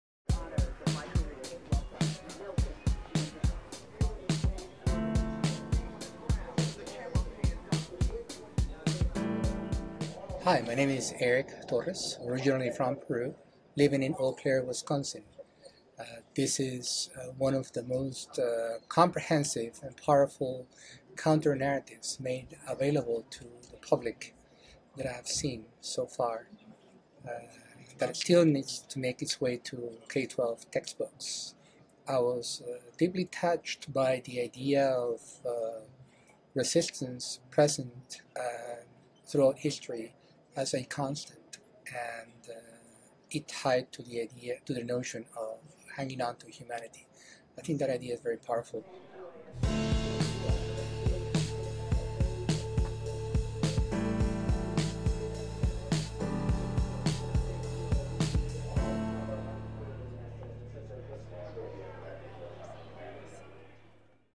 VisitorVoices - Hanging Onto Our Humanity
The Visitor Voices campaign highlights the Museum’s community of visitors and reflects their stories. Audiences are invited to share aspects of their visit—to empower them to see themselves as part of the museum’s history and community. Visitors are able to record their experience in our Reflections Booths located in our history galleries, or are invited to share via a social media staffer.

So to thank our incredible community of visitors, we are celebrating our anniversary month with a new series called VisitorVoices.

30 days. 30 voices. 30 stories. A Nation's Story.

Have you visited the museum? Tell us about your experience in the comment section!